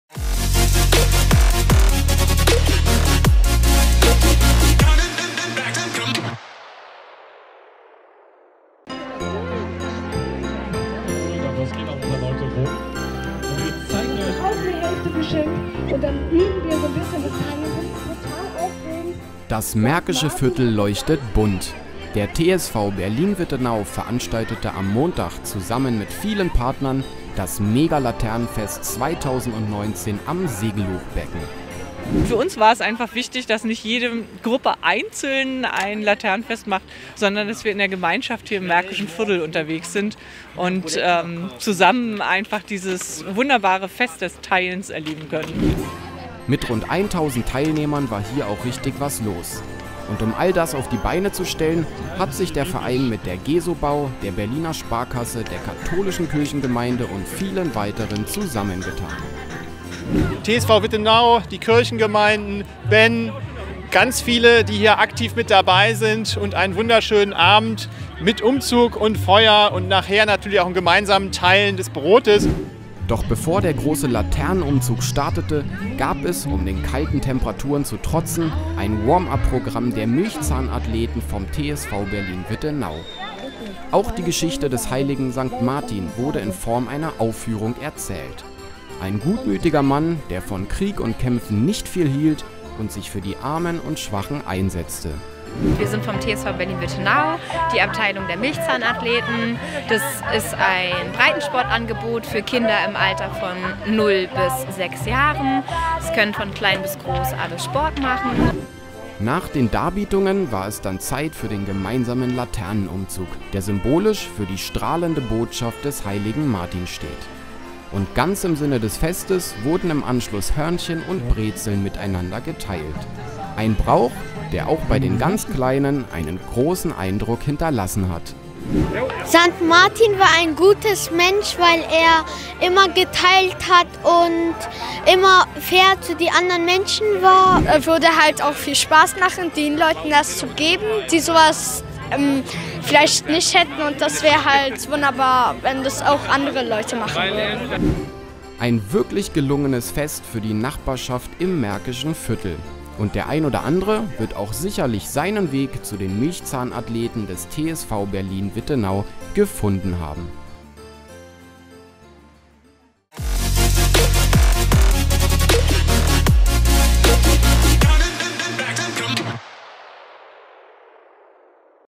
Großes Laternenfest 2019 des TSV Berlin-Wittenau
Gemeinsam feiern macht einfach mehr Spaß!
Der TSV Berlin-Wittenau veranstaltete zu Sankt Martin, zusammen mit vielen Partnern, einen großen Laternenumzug am Seggeluchbecken.
Ein wirklich gelungenes Fest für die Nachbarschaft im Märkischen Viertel.